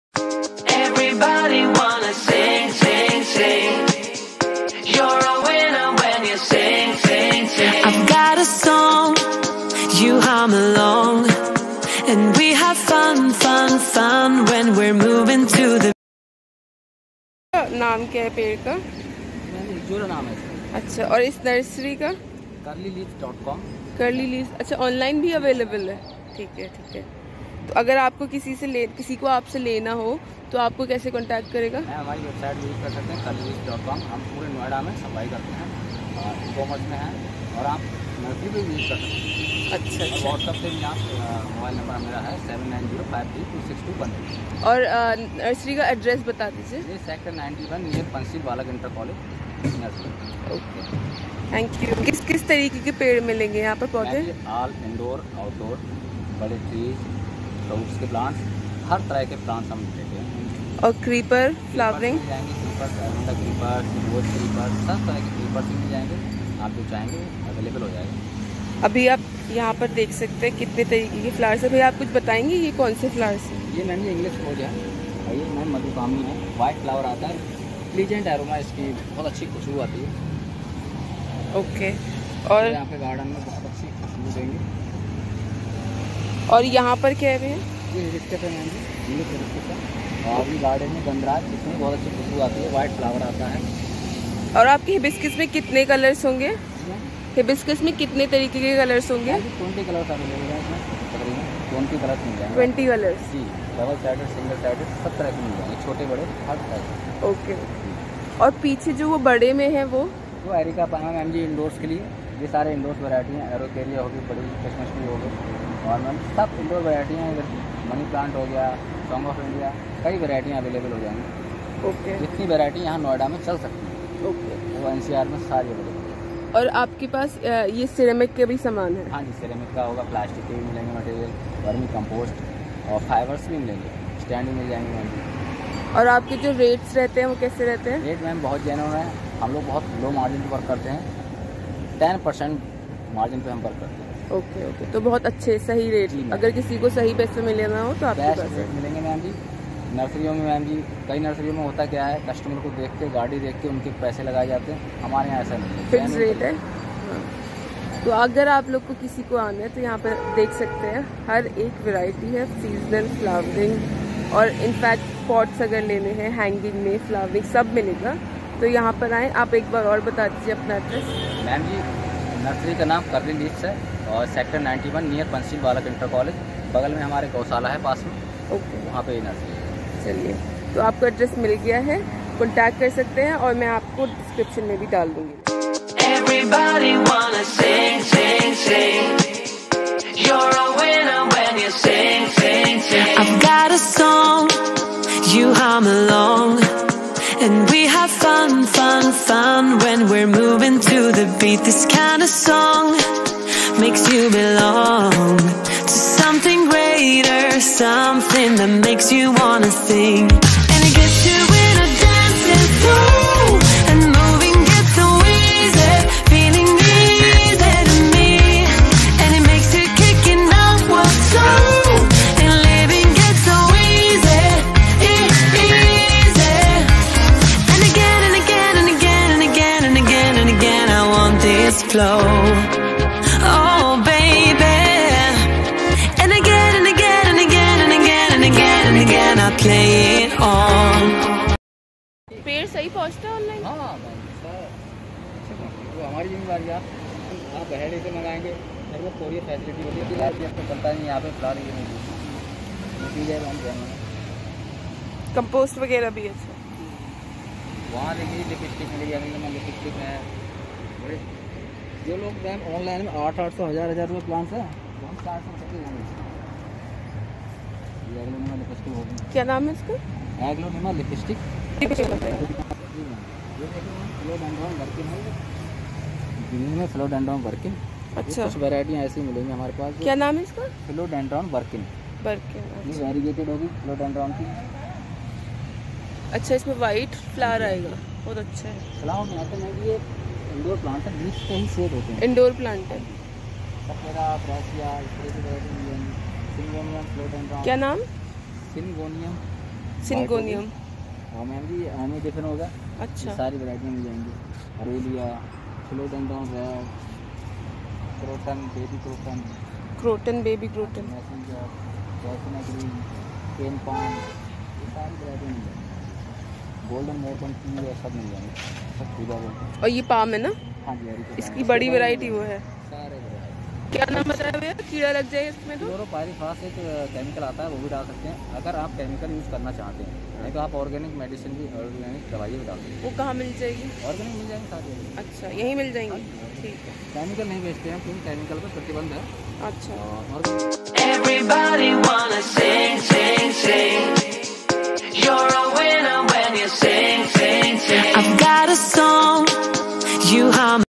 Noida Nursery Tour || Seasonal plants 🪴 shopping haul
This is famous and genuine nursery in Noida. Almost all variety of plants is available here. Resident of NCR Or Noida can go and check there. you can contact on website curli leaf.com. plz contact him.